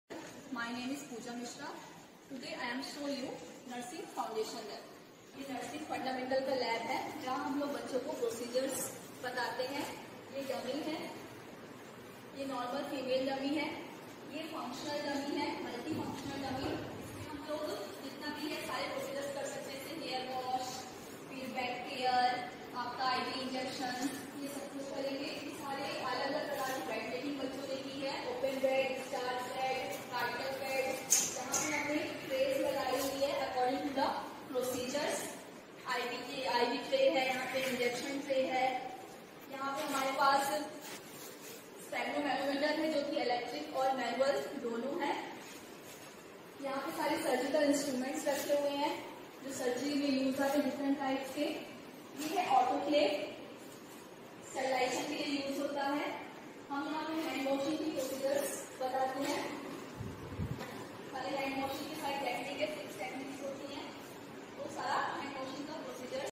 Orientation of fundamental lab
Orientation of fundamental lab and Introduce all articles and dummy and procedures in faculty of nursing department...
Faculty of Nursing Bhabha university, Bhopal.